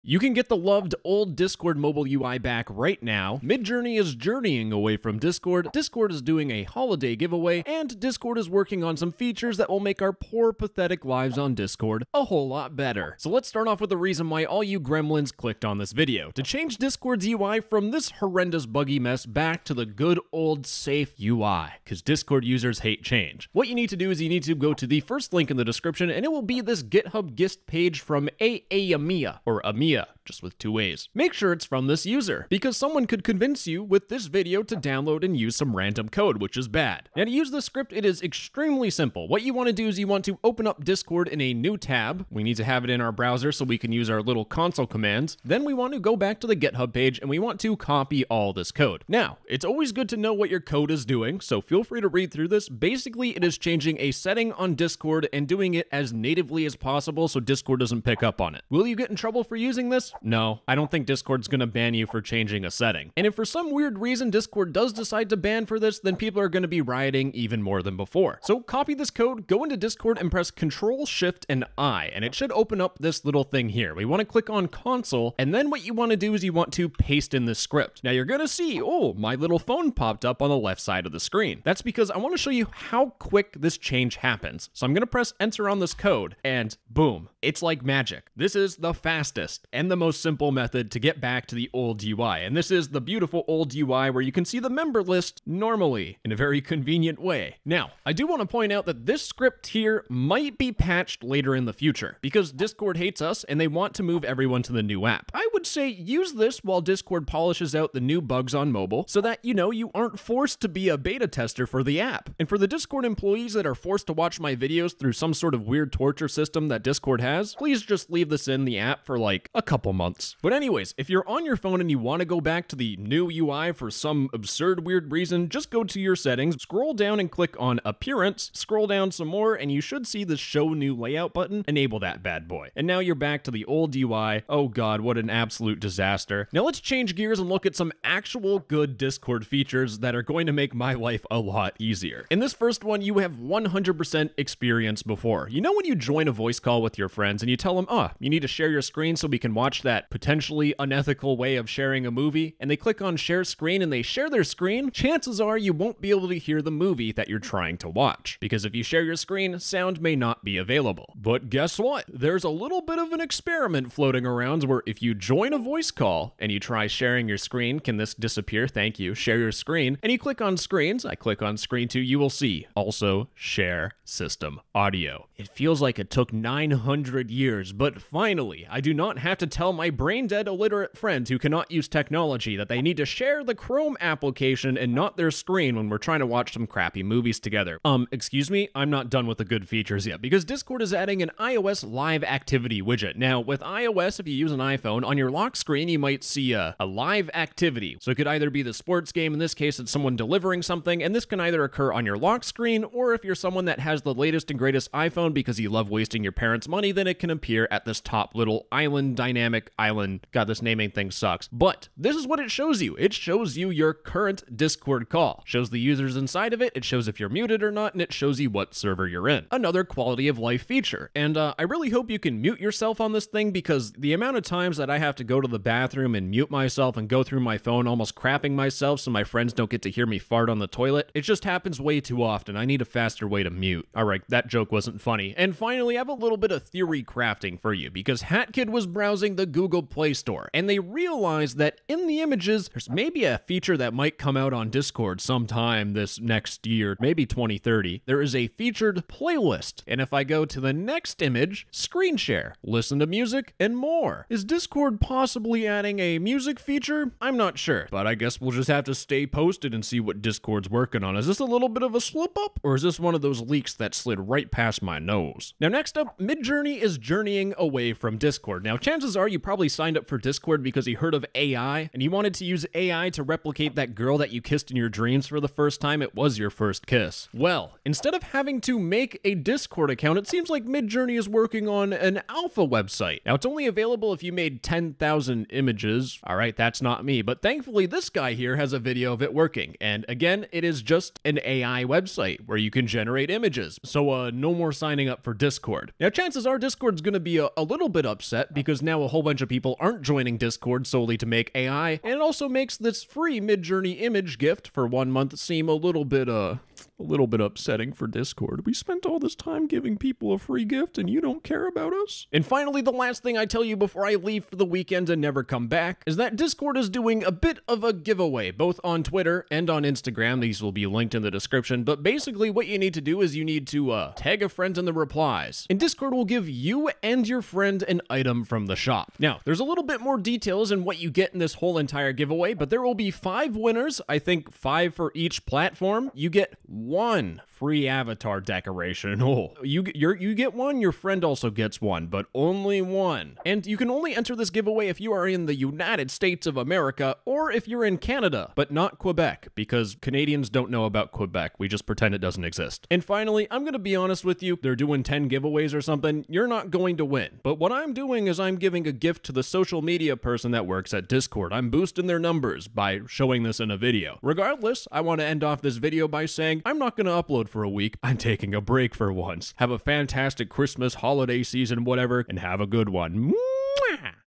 [PATCHED] Get Discord’s Old Mobile UI Back & Discord News!
Discord's new mobile update is not well received by people. A lot of people hate the new UI and wish they could go back to the safe and comfortable old UI. WELL GUESS WHAT, MERRY EARLY CHRISTMAS BOYO I GOT YOU.

Using a bit of technical Discord nerdiness, you can turn back the old UI. Now I don't expect this to work forever (nothing gold can stay), so use this while you can and avoid the buggy mess that is the mobile update.

Now it's Friday, that means that this is a Discord news video. And despite Discord employees cozying up for the holidays, we still have some heat Discord experiments coming out that make life easier for both Discord mods like myself, but also normal Discord users. Also some developments on Midjourney leave questions about the future of the Discord-based AI art generator. And finally, Discord gives us all a last sendoff with a winter giveaway.

TIMESTAMPS
00:00 - Old Mobile UI Tutorial
02:16 - GOOD DISCORD FEATURES?!1?!1?!
04:25 - Midjourney's Journey Away from Discord
05:12 - Discord Giveaway